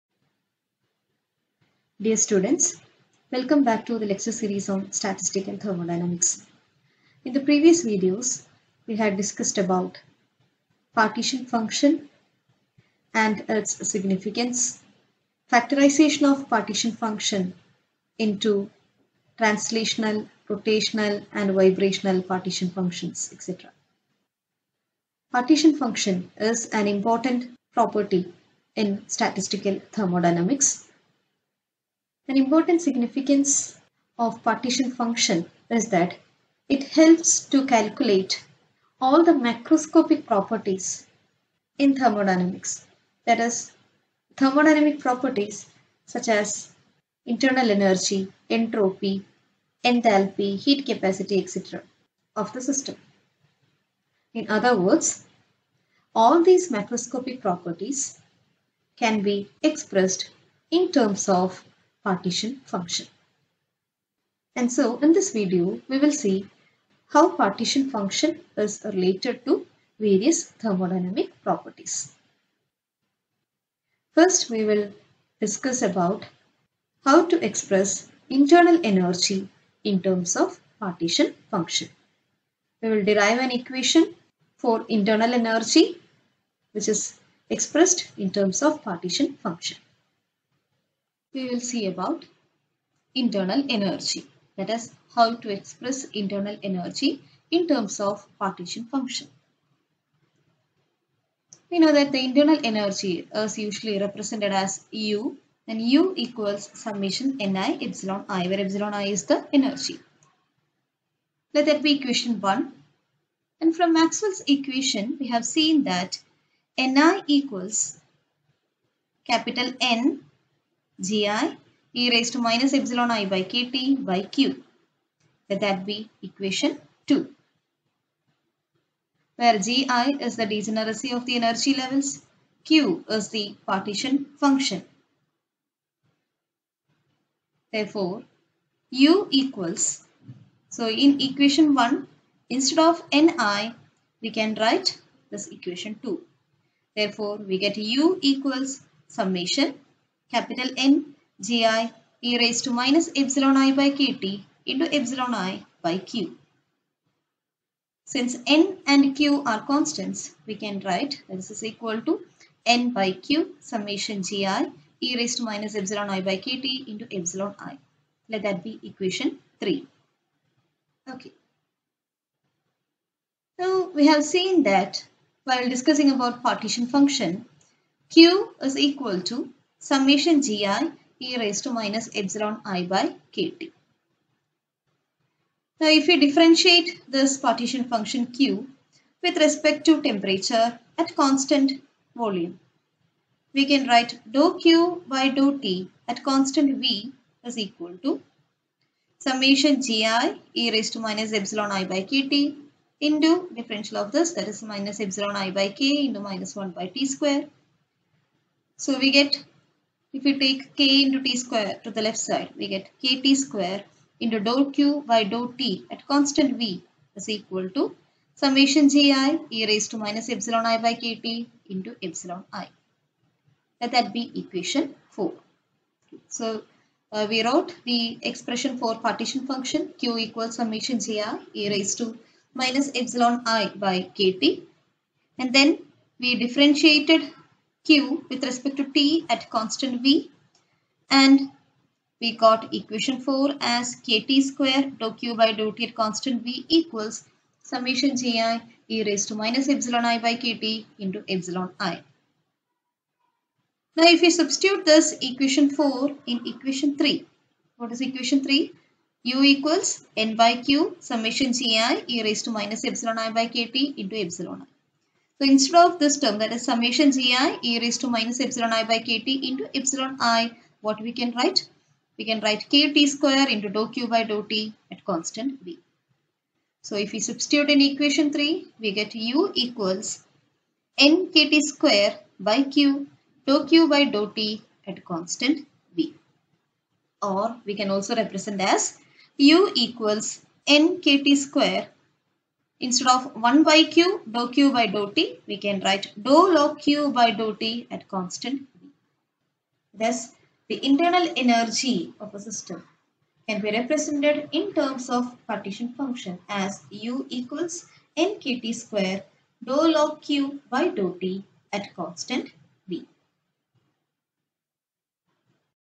Statistical Thermodynamics-Internal energy and Partition function
How to derive equation for internal energy in terms of partition function